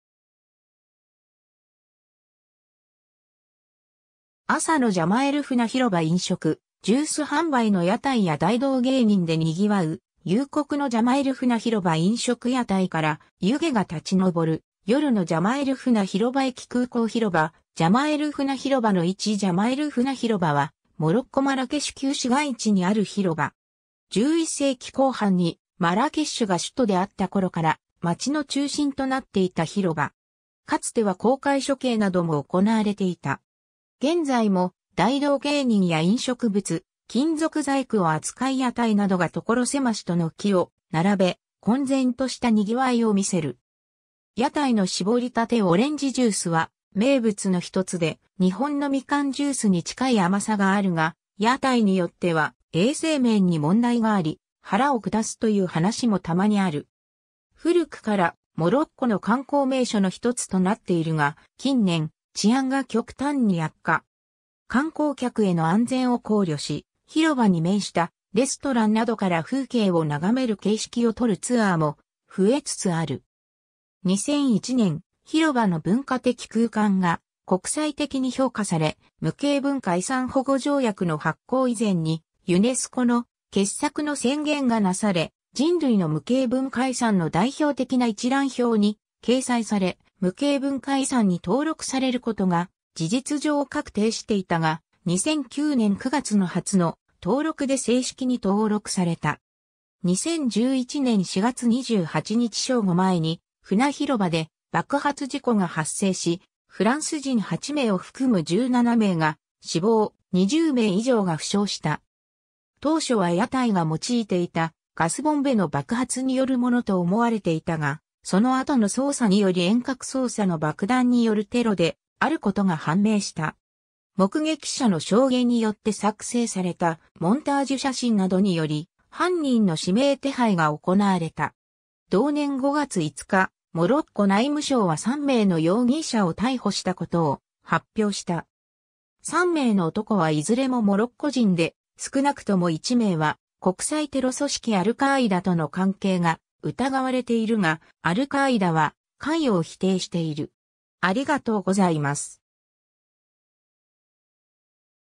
ジャマ・エル・フナ広場
朝のジャマ・エル・フナ広場 飲食・ジュース販売の屋台や大道芸人で賑わう、夕刻のジャマ・エル・フナ広場 飲食屋台から湯気が立ち上る、夜のジャマ・エル・フナ広場 駅 空港 広場 ジャマ・エル・フナ広場の位置（マラケシュ） ジャマ・エル・フナ広場（ - ひろば、英: Jemaa el-Fnaa）は、モロッコ・マラケシュ旧市街地にある広場。
 11世紀後半にマラケッシュが首都であった頃から街の中心となっていた広場。
かつては公開処刑なども行われていた。
現在も、大道芸人や飲食物、金属細工を扱い屋台などがところ狭しと軒を並べ、混然とした賑わいを見せる。
 屋台の絞りたてオレンジジュースは名物のひとつで、日本のみかんジュースに近い甘さがあるが、屋台によっては衛生面に問題があり、腹を下すという話もたまにある。
 古くからモロッコの観光名所の一つとなっているが、近年、治安が極端に悪化。
観光客への安全を考慮し、広場に面したレストランなどから風景を眺める形式を採るツアーも増えつつある。
 2001年、広場の文化的空間が国際的に評価され、無形文化遺産保護条約の発効以前にユネスコの「傑作の宣言」（第1回）がなされ「人類の無形文化遺産の代表的な一覧表」に掲載され、無形文化遺産に登録されることが事実上確定していたが、2009年9月の初の登録で正式に登録された。
 2011年4月28日正午前にフナ広場で爆発事故が発生し、フランス人8名を含む17名が死亡、20名以上が負傷した。
当初は屋台が用いていたガスボンベの爆発によるものと思われていたが、その後の捜査により遠隔操作の爆弾によるテロであることが判明した。
目撃者の証言によって作成されたモンタージュ写真などにより犯人の指名手配が行われた。
同年5月5日、モロッコ内務省は3名の容疑者を逮捕したことを発表した。
3名の男はいずれもモロッコ人で、少なくとも1名は国際テロ組織アル・カーイダとの関係が疑われているが、アル・カーイダは関与を否定している。